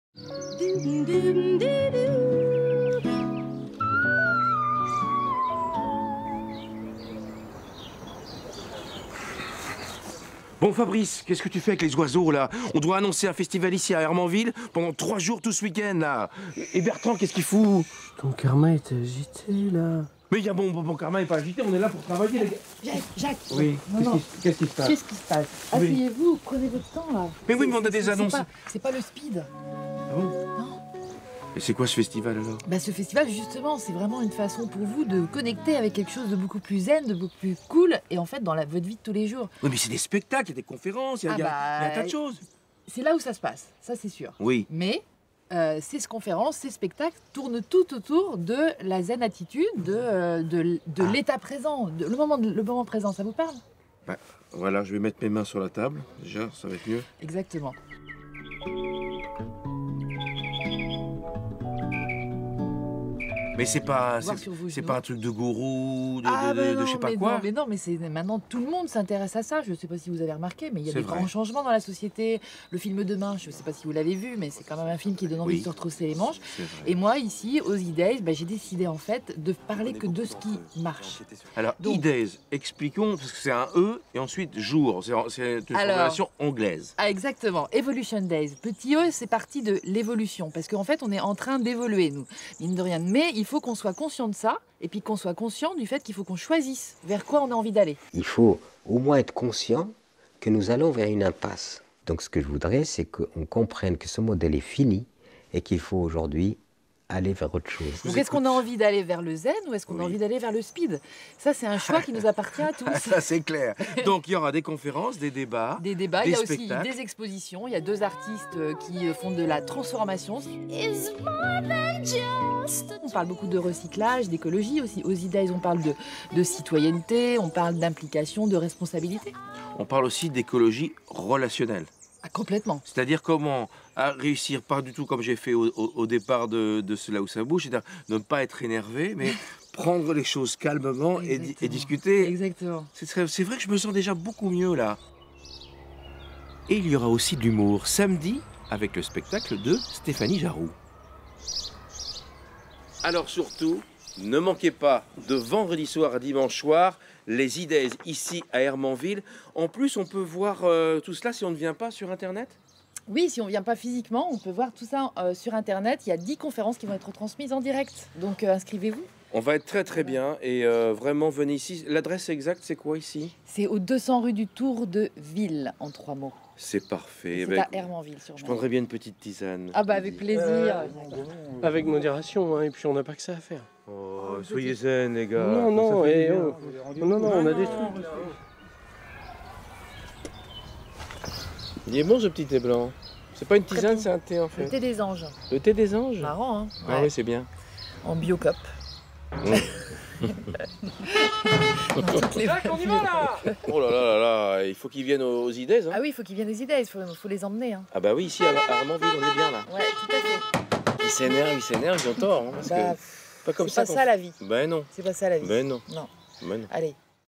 LOCB du 26 mai 2016: zen attitude avec le festival eDays à Hermanville
Dans ce monde plein de tensions, l'équipe de LOCB vous propose de prendre un grand bol de coll et d'adopter la zen attitude avec le festival eDays. Au programme: des conférences et des spectacles de vendredi à dimanche pour réfléchir au monde de demain, sans s'énerver.